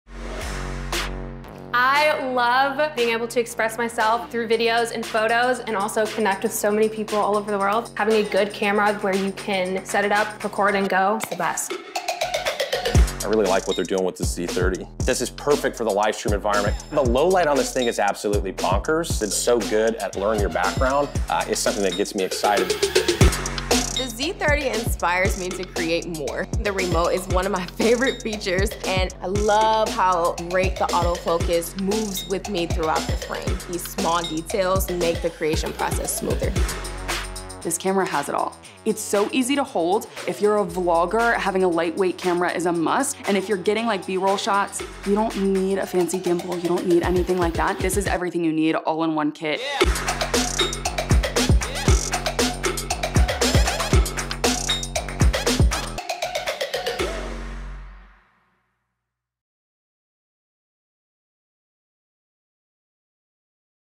New Nikon Z 30 – The Vlogger, Creator, Streamer Camera
Creators, meet the camera you’ve been waiting for. The Nikon Z 30 is packed with the features you need: a front-facing touch screen, crisp 4K video with beautifully blurred backgrounds, worry-free autofocus, low light capabilities so you can create anywhere, up to 125 minutes of continuous video recording time, a red REC light so you always know when you're shooting and so much more! Vloggers, streamers and creators like @BrittanyLupton , @CathrinManning , @ErinOnDemand , @LEGIQN show us what they love about the new Nikon Z 30.

Bundle a Z 30 lens kit with the Creator Accessory Kit, and you’ll get everything you need to start creating: camera, lens, SmallRig Tripod Grip, Bluetooth remote control and Rode VideoMicro microphone.

Get the details about the Nikon Z 30 and Creator Accessory Kit